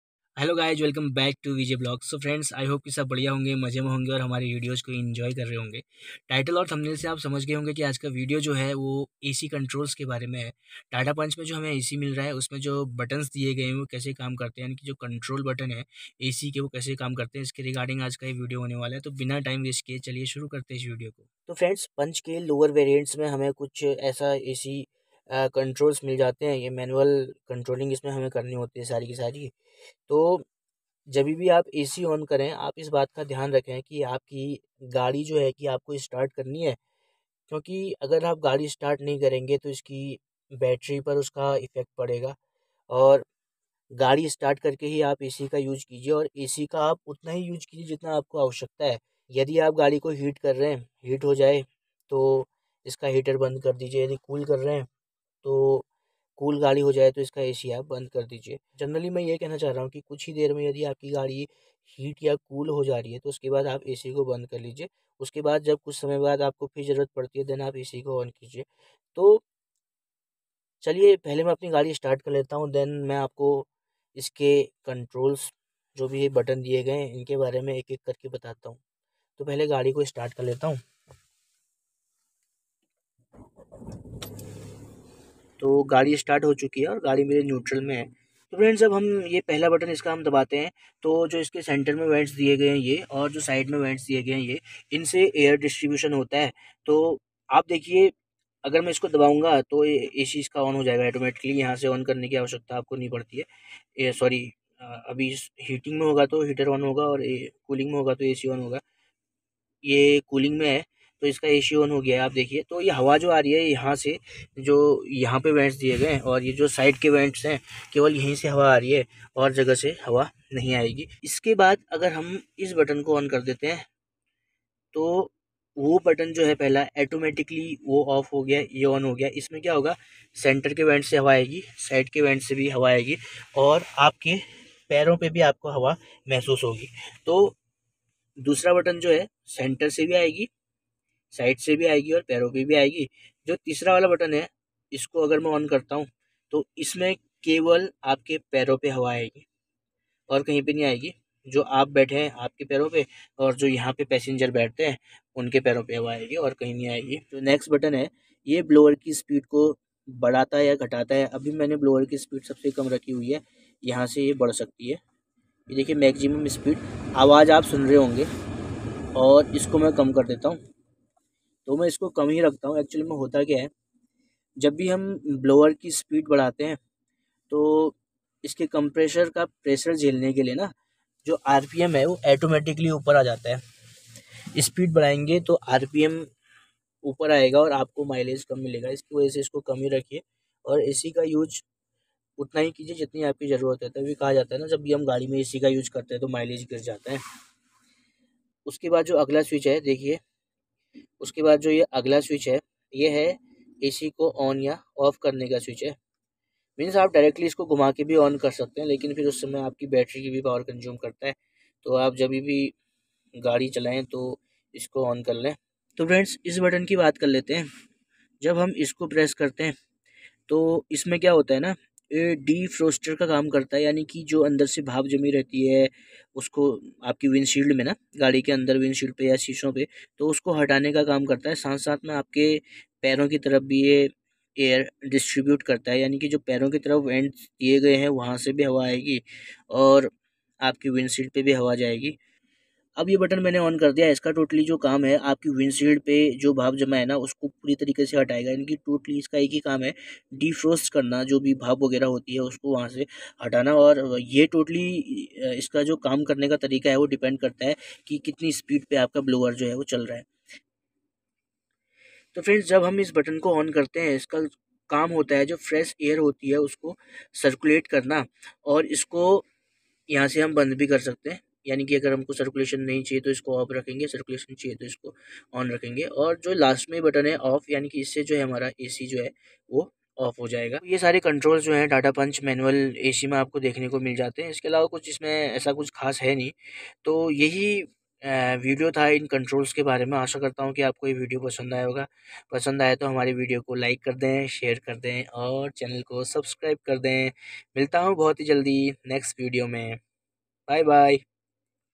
TATA PUNCH 🔥🔥 आइए जानते हैं punch के AC-CONTROLS कैसे काम करते हैं?
आइए जानते हैं punch के AC-CONTROLS कैसे काम करते हैं?